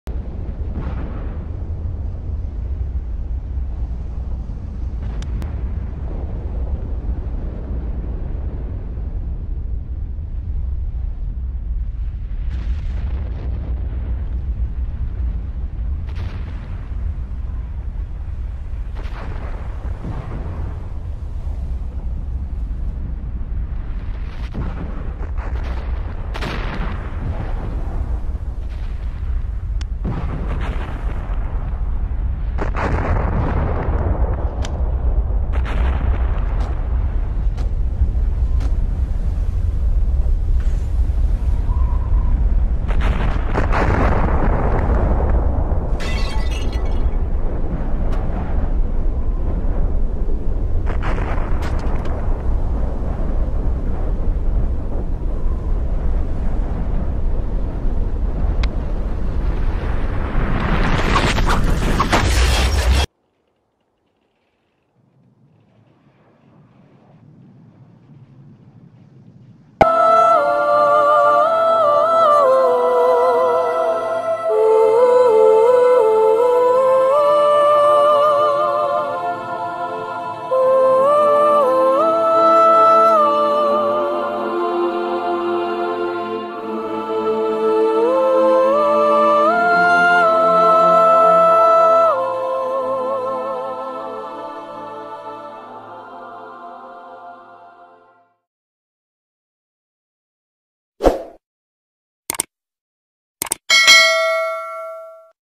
Tonga volcano eruption fills sky with black ash clouds
Tonga volcano eruption fills sky with black ash clouds people lost home.mp4